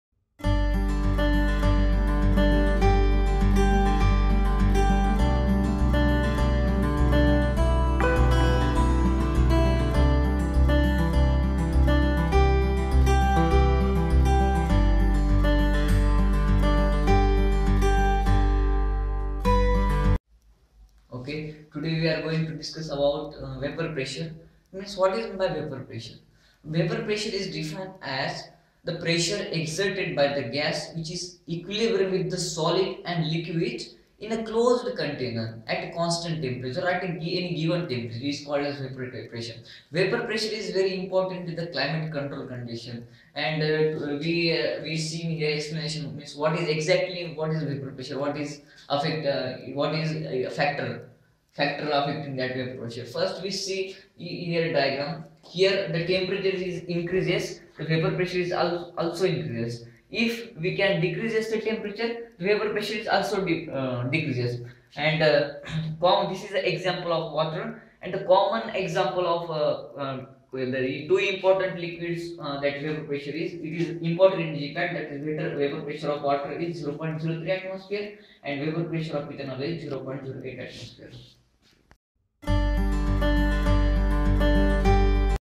States of matter-Vapour pressure
Vapor pressure or equilibrium vapour pressure is defined as the pressure exerted by a vapor in thermodynamic equilibrium with its condensed phases (solid or liquid) at a given temperature in a closed system. The equilibrium vapor pressure is an indication of a liquid's evaporation rate.